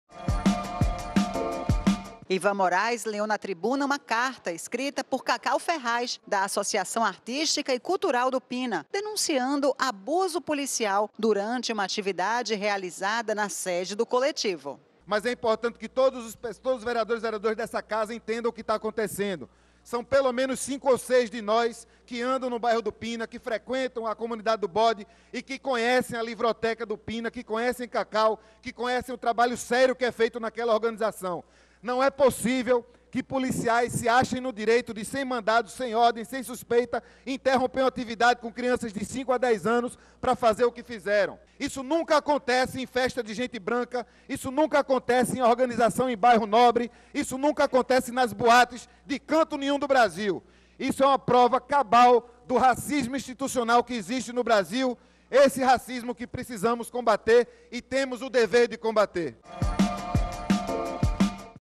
Ivan Moraes lê carta contra abuso policial
Ivan Moraes leu na tribuna uma carta escrita por Cacau Ferraz, da Associação Artística e Cultural do Pina, denunciando abuso policial durante uma atividade realizada na sede do coletivo.